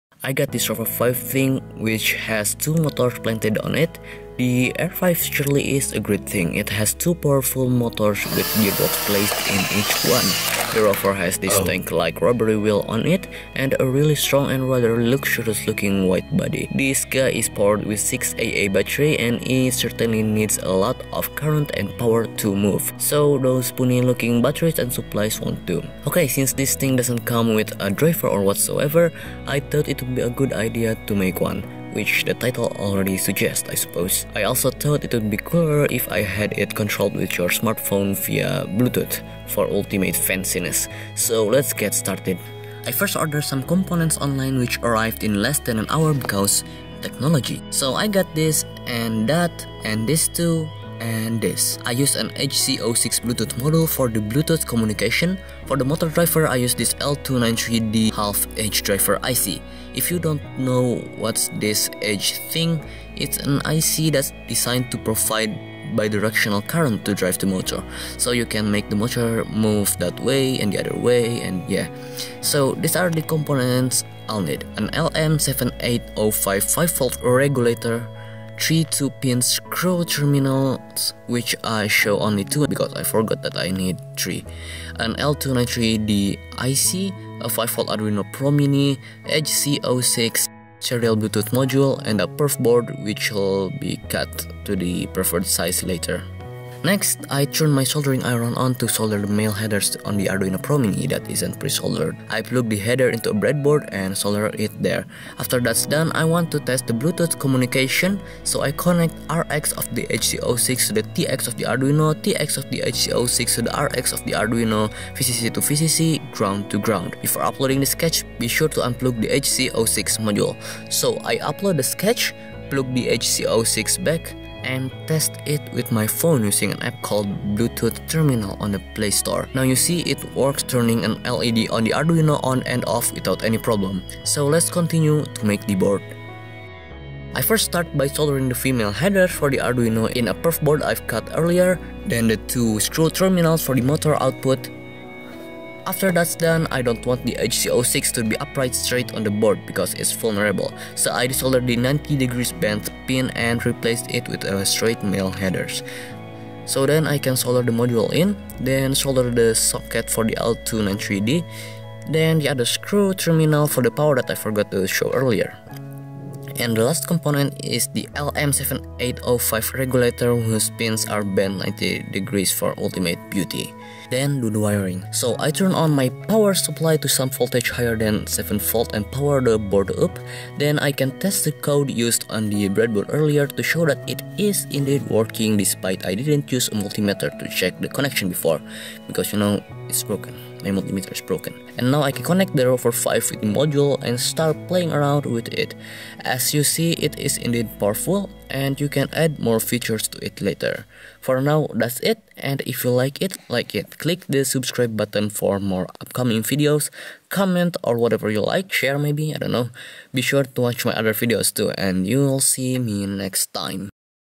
Rover 5 Bluetooth Driver - With HC-06, L293D, Arduino Pro Mini
The Rover 5 is a powerful robot platform, with its weight and power, this thing is not a toy. This is a heavy-duty platform used to make great projects; I made a Bluetooth controlled driver for it in this video.
There's nothing so something in this video, but currently, I don't have any ideas in my mind. So I'll do what I think I can do to make this thing move.

Anyway, for the video quality, I've learned a lot about audio processing and recording this past month, and I think I know something more now. So enjoy this new audio quality and new music track, and hopefully I can buy a real, low-noise, somewhat professonal microphone for the next video.